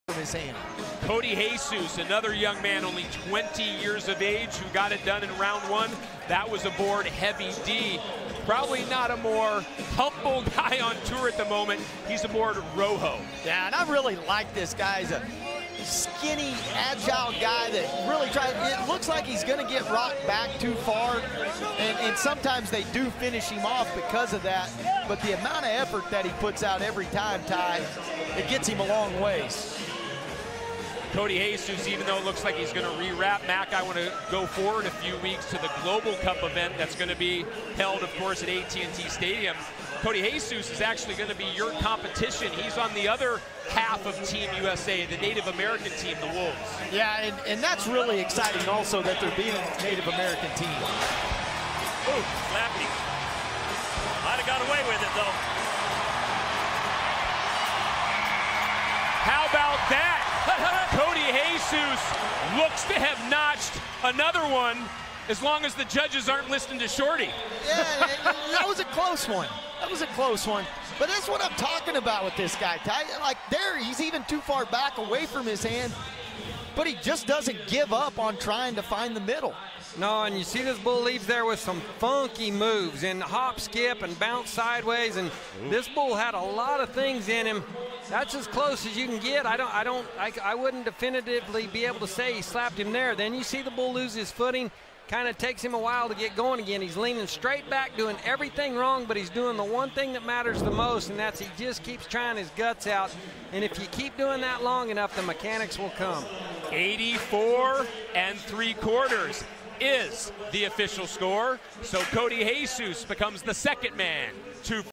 Cody Jesus rides Rojo for 84.75 points (PBR)
Cody Jesus rides Rojo for 84.75 points in Round 2 of the 2019 PBR UTB Monster Energy Buckoff At The Garden Presented By Ariat in New York City, NY